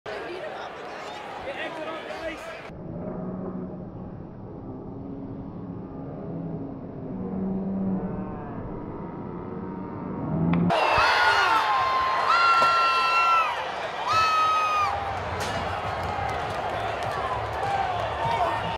Roope Hintz Overtime Game Winner vs San Jose Sharks | Dallas Stars March 5, 2024
How did Logan and Roope make that look so easy?